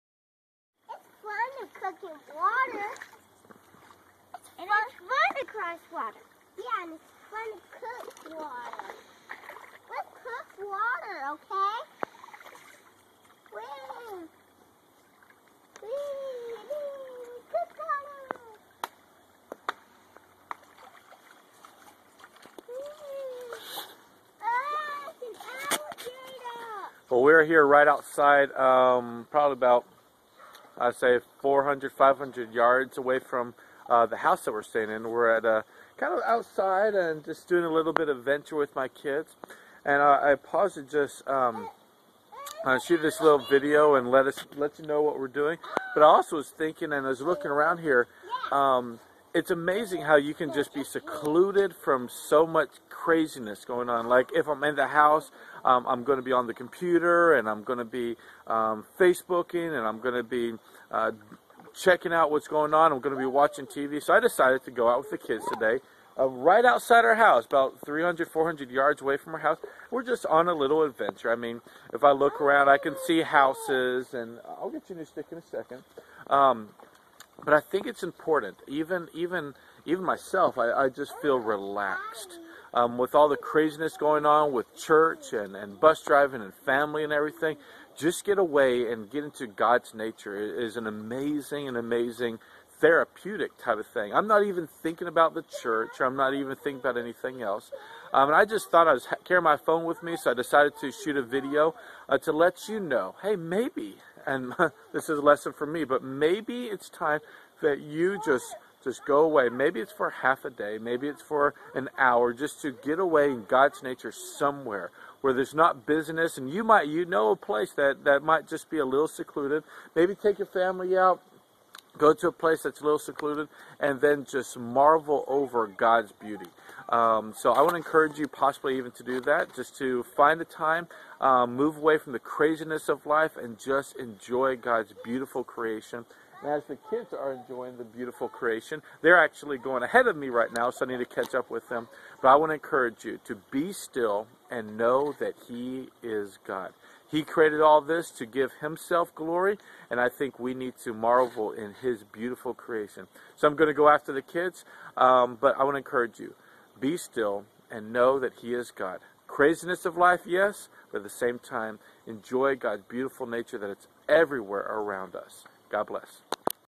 A walk in nature
Taking a break from the crazy life and heading out into GOD's amazing nature with my kids!